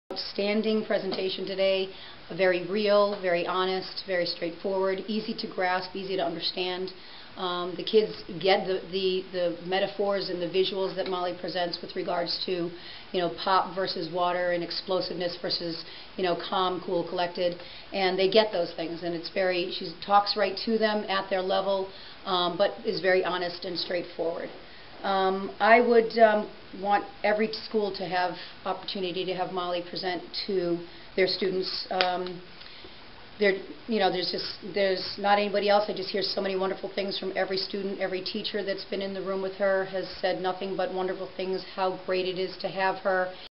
Youth Speaker Molly Kennedy Testimonial
Testimonial from Kenmore West HS Challenge Club Advisor, Darcy France-Boatman.